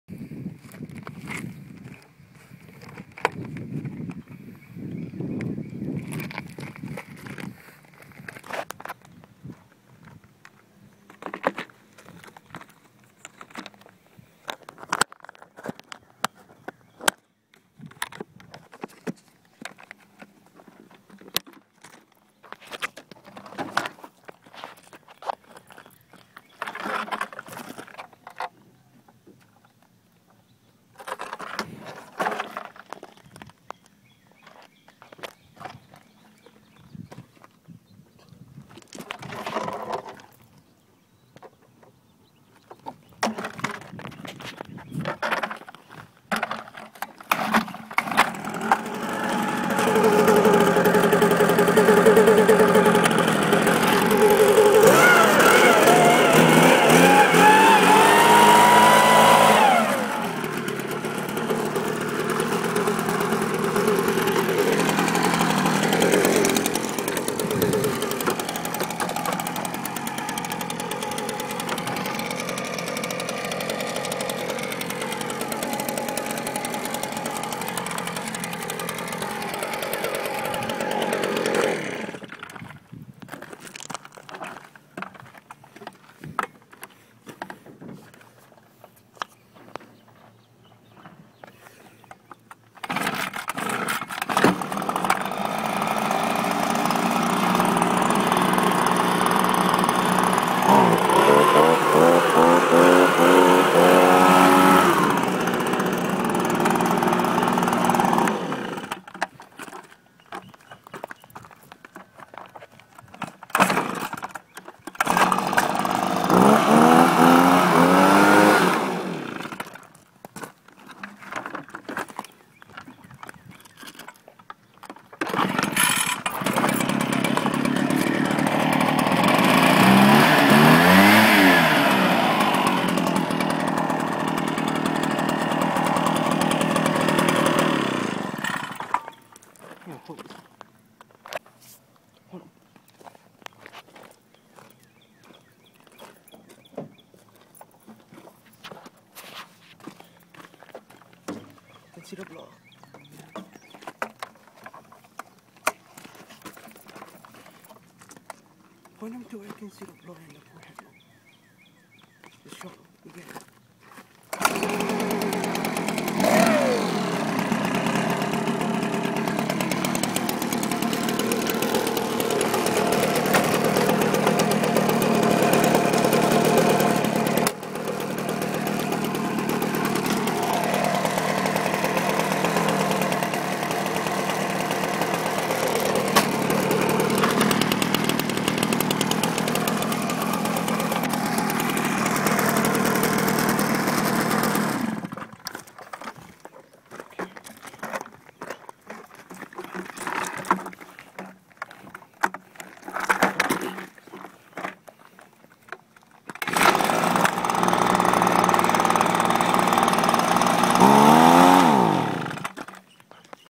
starting redmax hbz2601-CA and echo pb 250 and echo pb 620h blower
this is part of a video request for Smallgasengine1 you asked to see the air filter and you got it